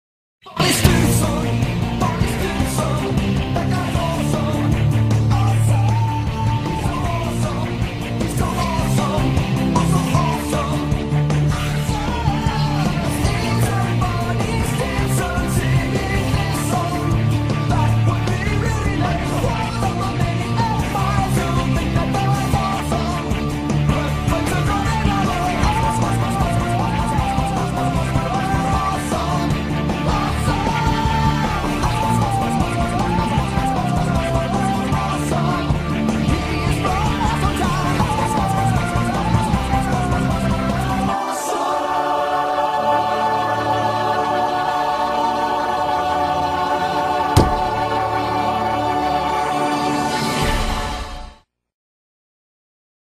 Neil Patrick Harris (Barney Stinson) graffiti speed painting
Barney Stinson graffiti I made with Facebook

no tablet was used, everything was drawn with a mousse